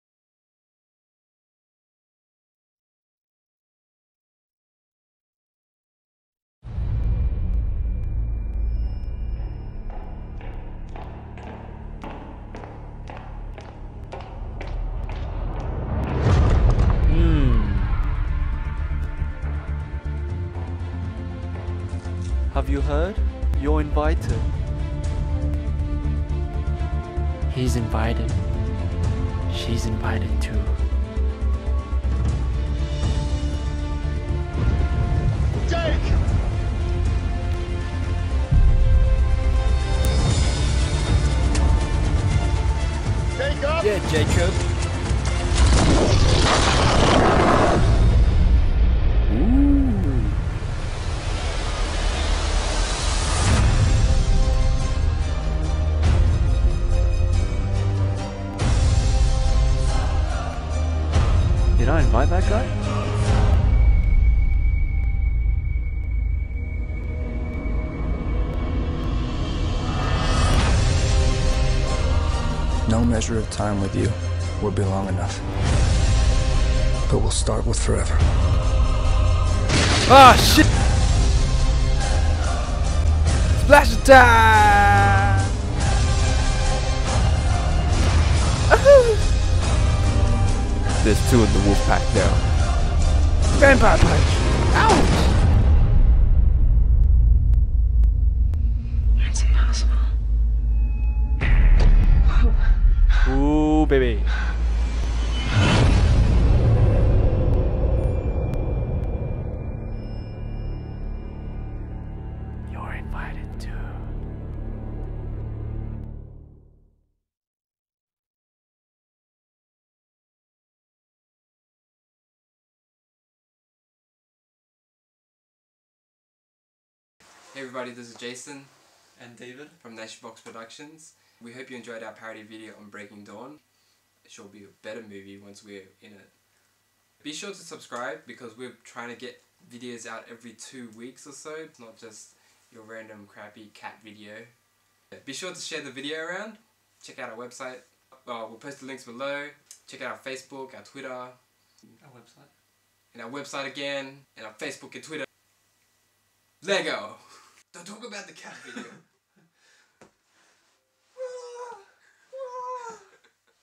THE TWILIGHT SAGA: BREAKING DAWN Part 1 - Teaser parody
Two extra characters in the final chapter of twilight [Breaking Dawn] get out of hand